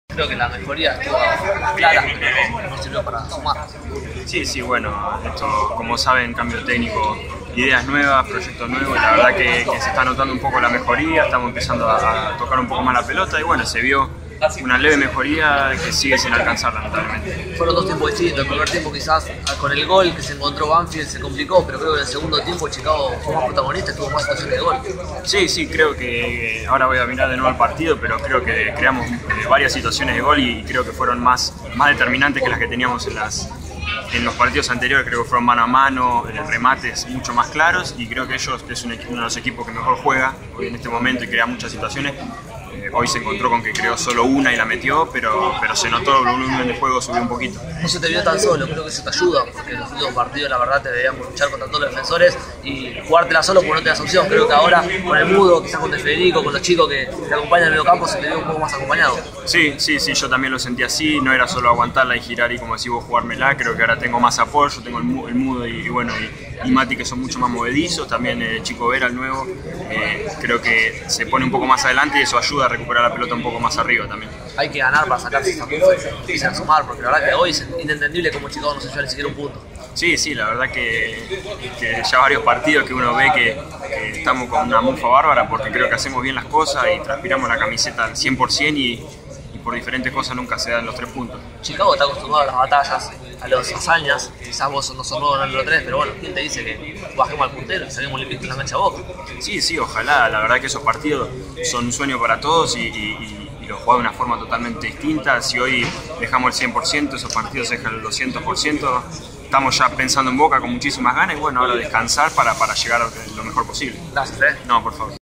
Nueva Chicago 1 - Banfield 2: Luis Soligniac
El Delantero de Nueva Chicago, Luis Soligniac, al término del partido en el que Banfield derrotó a Chicago por 2 a 1 en Mataderos.
Nota: Estaban Cavalotti / Video y Edición; Miguel Angel Scarcello